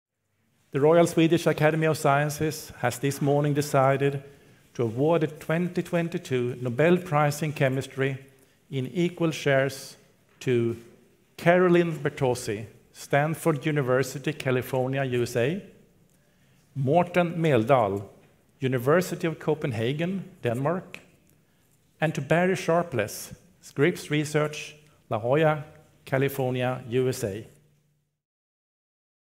Two Americans and a Dane win chemistry Nobel | AFP
The Nobel Chemistry Prize is awarded to a trio of chemists for laying the foundation for a more functional form of chemistry. Americans Carolyn Bertozzi and Barry Sharpless, together with Denmark's Morten Meldal are honoured for "for the development of click chemistry and bioorthogonal chemistry". The award marks the second Nobel for 81-year-old Sharpless, who won the chemistry Nobel in 2001. SOUNDBITE